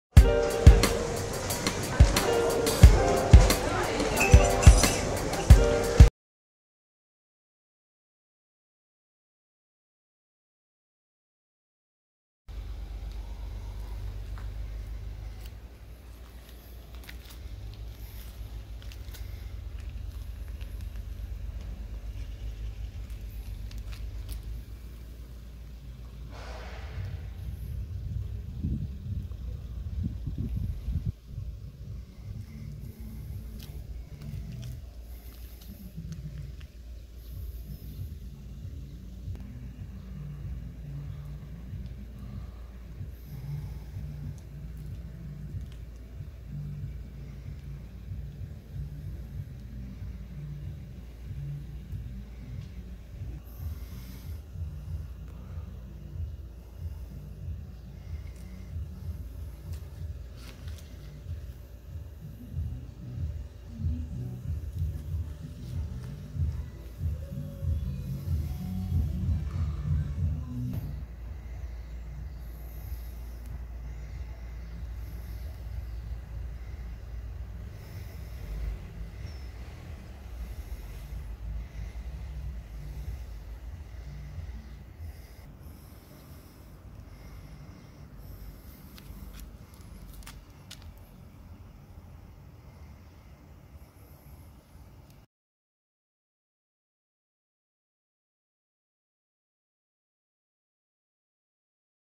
Dacia 1100 Overview
🆕️ Dacia 1100 spotted în Bucharest...
The car was for sale.